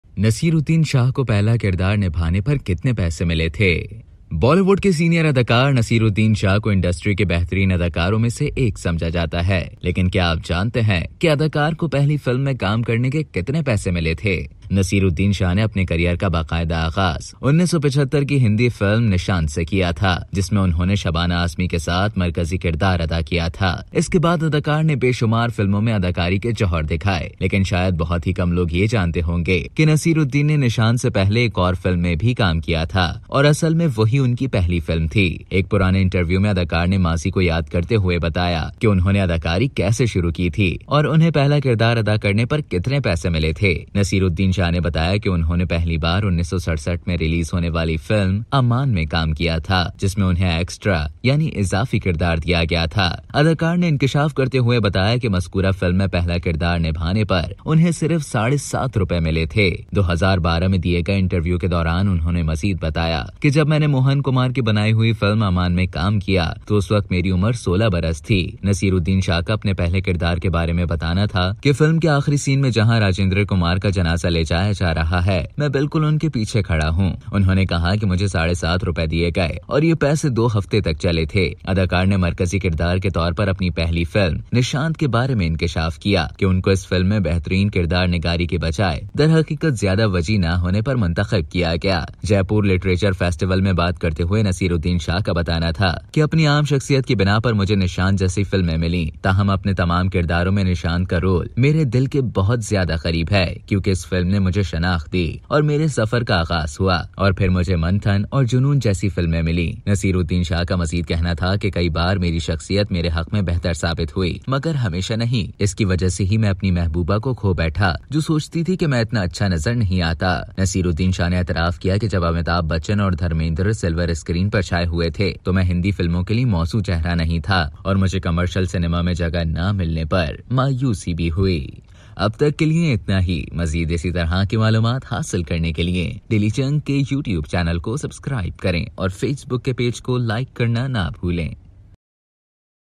"How much money was Naseeruddin Shah paid for his first role?"
"Naseeruddin Shah, one of Bollywood's senior actors, is considered among the finest actors in the industry. But do you know how much he was paid for his first film role?"